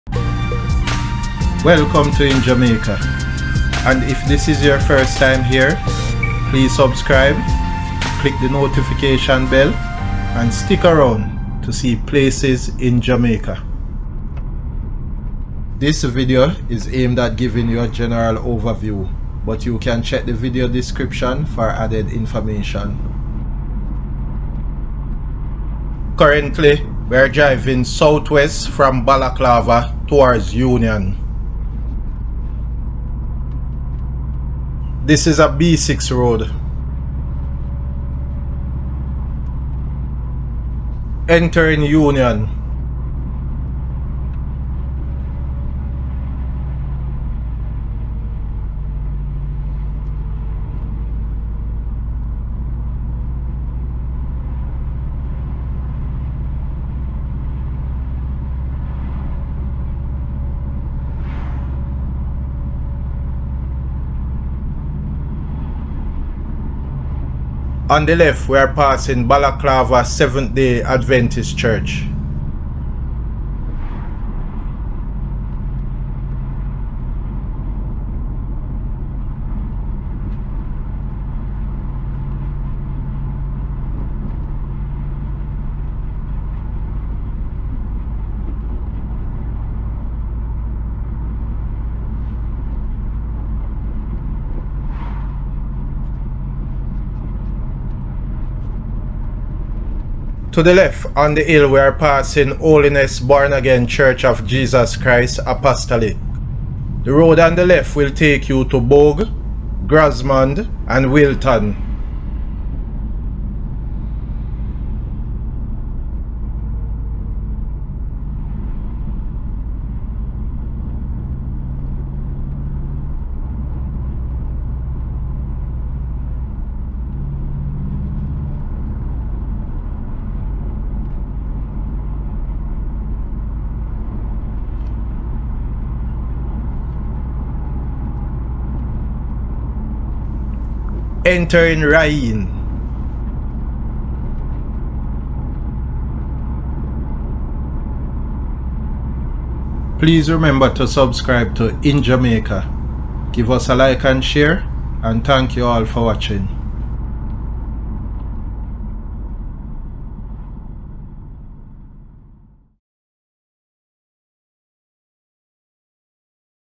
Union, St Elizabeth, Jamaica
Union is located in St Elizabeth, Jamaica (Caribbean).

⭐ Points of interest:
0:00 Intro
00:25 Balaclava
00:34 B6 Road
00:40 Union
01:07 Balaclava Seventh-Day Adventist Church
01:40 Holiness Born Again Church of Jesus Christ, Apostolic
01:46 To Bogue
01:46 To Grosmond
01:46 To Wilton
02:21 Raheen

⭐ Jamaica Road Network:
Main roads in Jamaica are assigned a letter and number for a designated route.
A - Primary (or Principal) Road.
B - Secondary Road.
T - Highways (Toll Roads)

LOCATION:
2021 Jamaica Road Trip